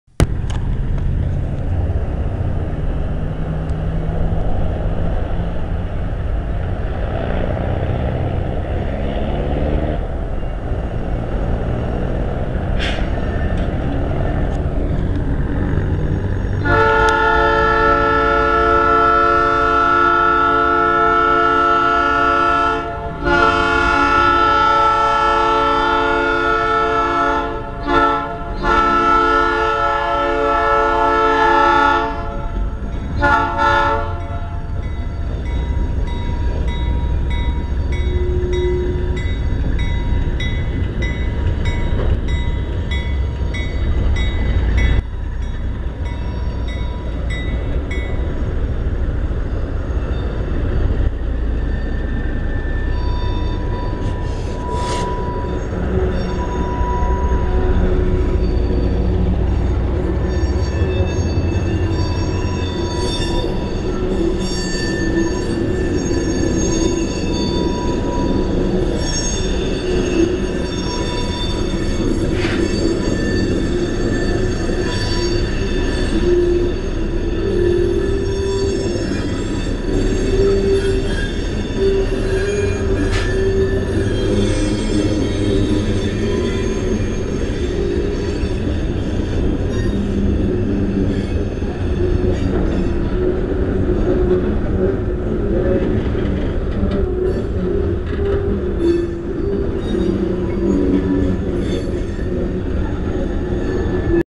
Ferrovalle #2101 y #2100 saliendo de Almacenadora Sur
Sacando no se cuantas tolvas cargadas de grano.